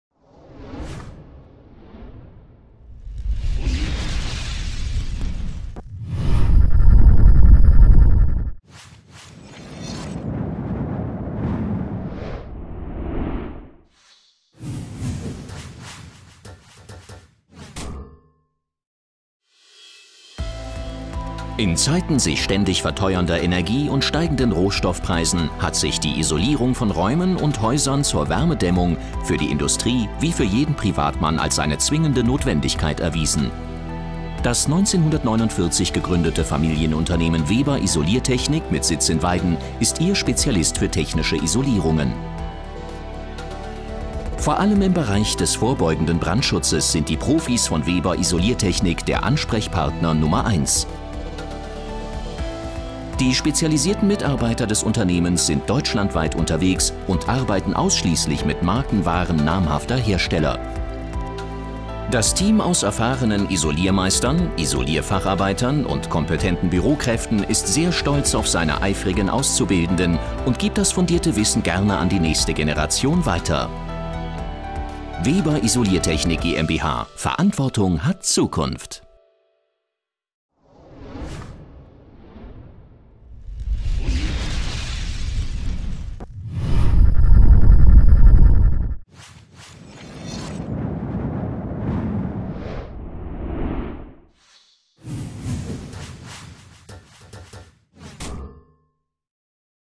Weber Isoliertechnik | Unternehmensfilm
In Zeiten sich ständig verteuernder Energie und steigenden Rohstoffpreisen hat sich die Isolierung von Räumen und Häusern zur Wärmedämmung für die Industrie wie für jeden Privatmann als eine zwingende Notwendigkeit erwiesen.

Das 1949 gegründete Familienunternehmen Weber Isoliertechnik mit Sitz in Weiden ist Ihr Spezialist für Technische Isolierungen.

Vor allem im Bereich des vorbeugenden Brandschutzes sind die Profis von Weber Isoliertechnik der Ansprechpartner Nummer Eins. Die spezialisierten Mitarbeiter des Unternehmens sind deutschlandweit unterwegs und arbeiten ausschließlich mit Markenwaren namhafter Hersteller.

Das Team aus erfahrenen Isolierer Meistern, Isolierfacharbeiter und kompetenten Bürokräften ist sehr stolz auf seine eifrigen Auszubildenden und gibt das fundierte Wissen gerne an die nächste Generation weiter.

Weber Isoliertechnik GmbH – Verantwortung hat Zukunft!